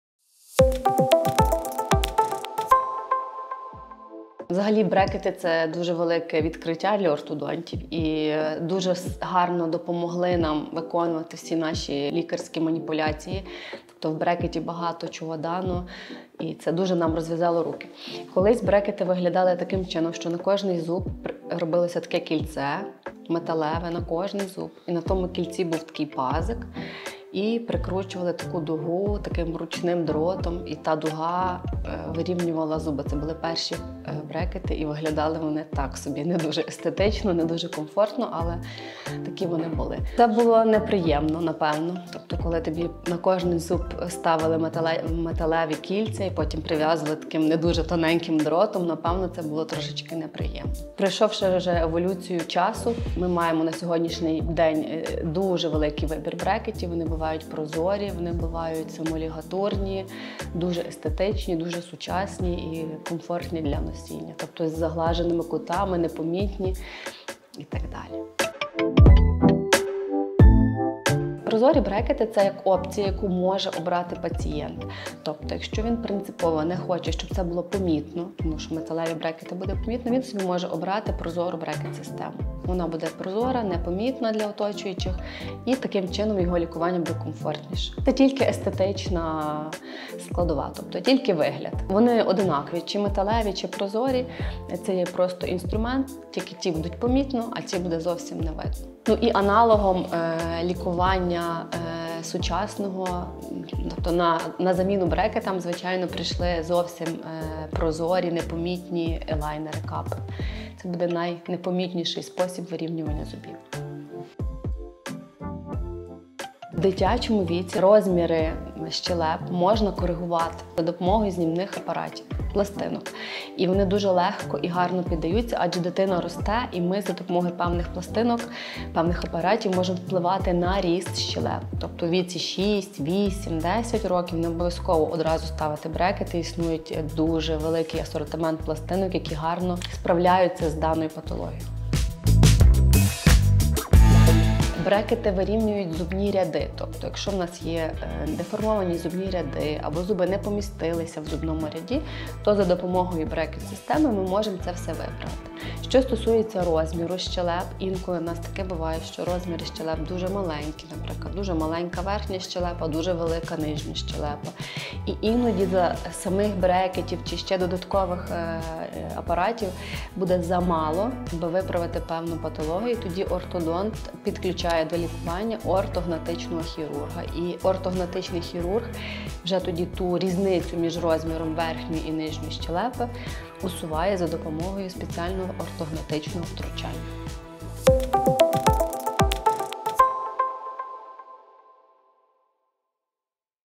Як виглядали брекети в минулому та що таке прозорі брекети: цікаві факти від ортодонта.
стоматолог_рекомендує клініка родини Прощин